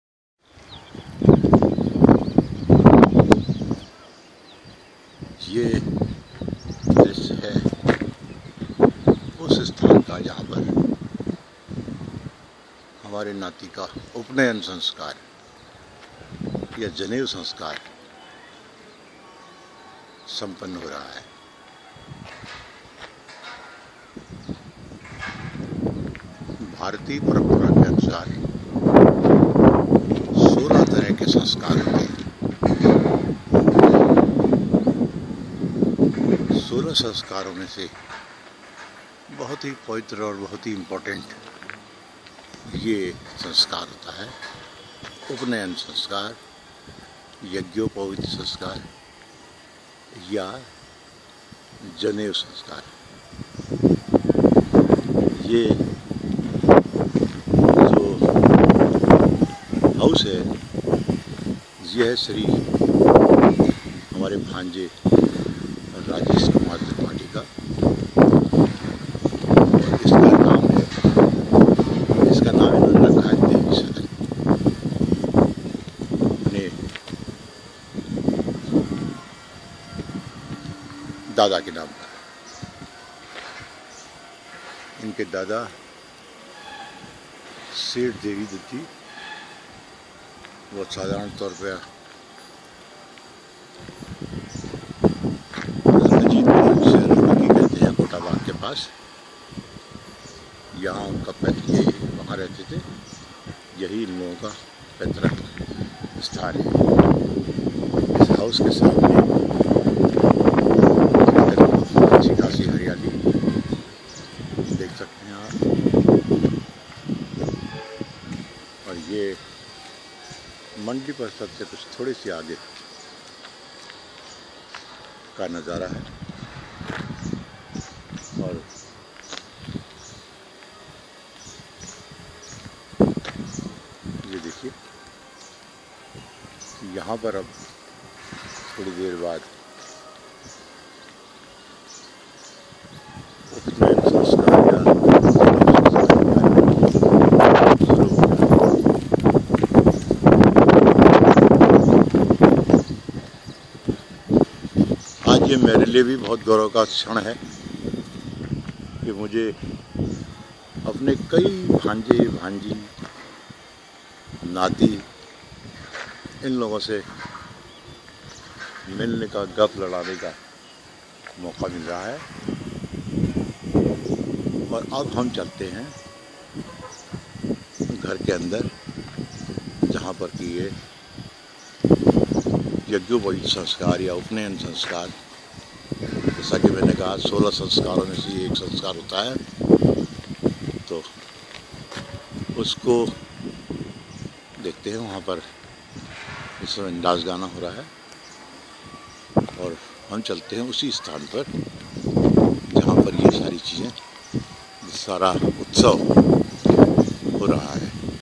6.1-Social Issues-Yagyopaweet Sanskar-Rituraj Son of Rajesh Tripathi
1 अप्रैल, 2015 को बड़ी खुशी दिन था ! इस दिन हमारे भांजे श्री राजेश त्रिपाठी के पुत्र ऋतुराज का यज्ञोपवीत संस्कार 'देवी निवास', गौजाजाली, हल्द्वानी मैं  संपन्न हवा  ! इस अवसरों मैं पूरे परिवार का मिलन भी हुवा ! मैं श्रीमती उदा बड़ोला के साथ हल्द्वानी गया  l वहाँ पर इस अवसर पर बड़ी संख्या मैं रिस्तेदारों के बीच  नई पुरानी बातों को याद किया गया l मुझे भी गर्व था कि मेरे नाती का संस्कार हर्शौल्लाश के साथ संपन्न हवा l राजेश ने दिल खोल कर इस समारोह को मनाया और हम सबने ही भांजे, भांजियों तथा अन्य रिश्तेदारों के साथ जम कर समारोह का लुत्फ़ उठाया !   हिन्दू धर्म मैं 16 संस्कारों मैं से यज्ञोपवीत संस्कार का बहुत  महत्व है l उत्तराखण्ड मैं इसे  जनेऊ संस्कार, उपनयन संस्कार, मुंडन संस्कार  आदि के नाम से  जाना जाता l जब बालक  की उम्र विषम संख्या मैं होने पर, अर्थात 13, 15, 17 आदि की होने पर ही यह संस्कार किया जाता है l हम परम पिता परमेश्वर से प्रार्थना करते हैं कि ऋतुराज फूलें फलें एवं अपने खानदान का नाम रोशन करें ! यही हमारी  कामना  है , यही हमारा आशीर्वाद सबको है !